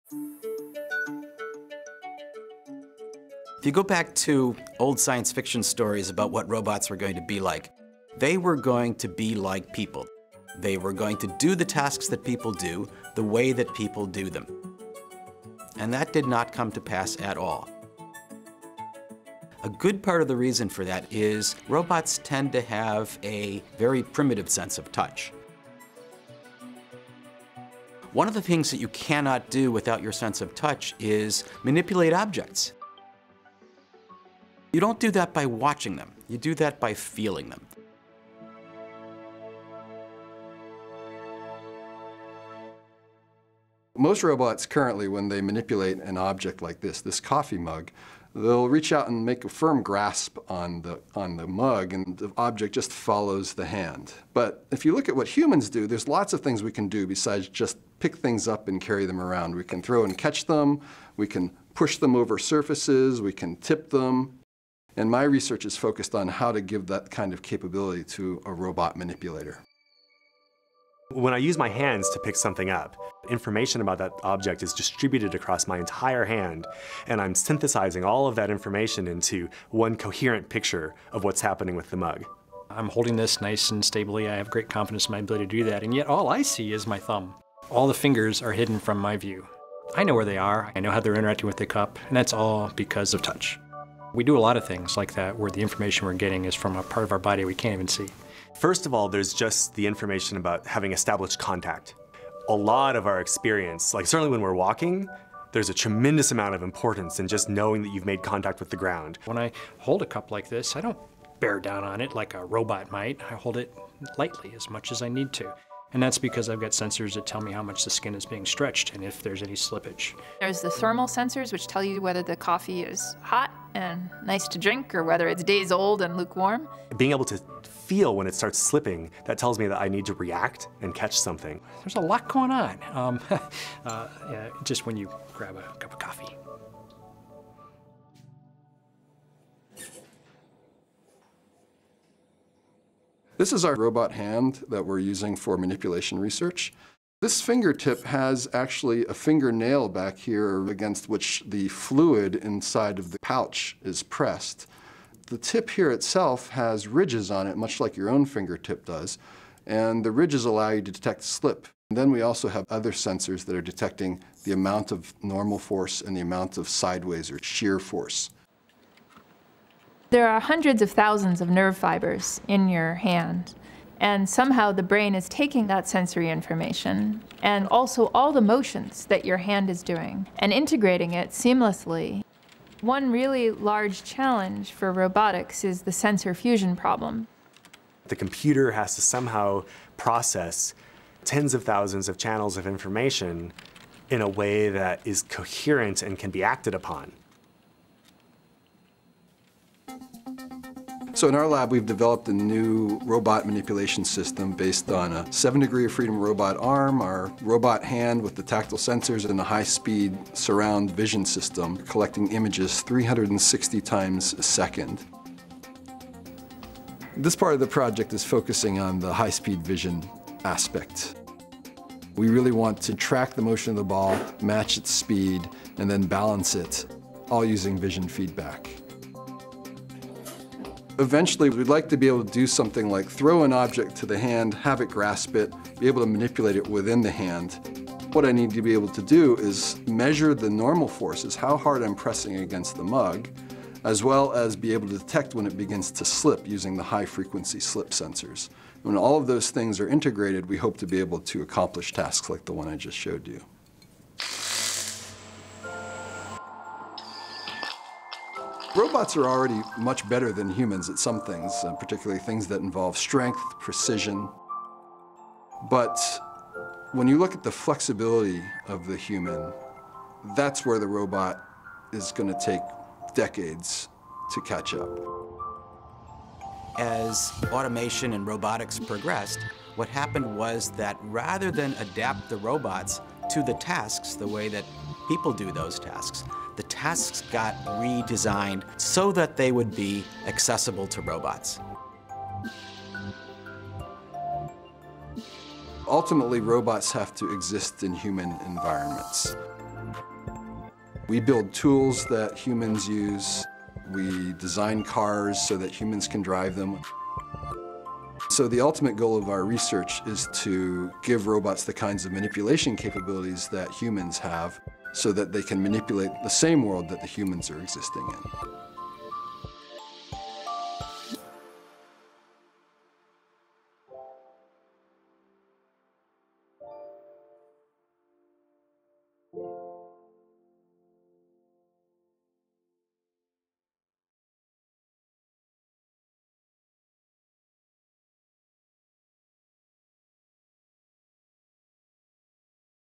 Engineering Touch
Engineers at Northwestern University’s McCormick School of Engineering are working to incorporate touch into robotics. Researchers have developed a new robotic manipulation system that uses tactile sensors and a high-speed camera vision system to catch and manipulate objects. The goal is to give robots the same manipulation capabilities as humans. ____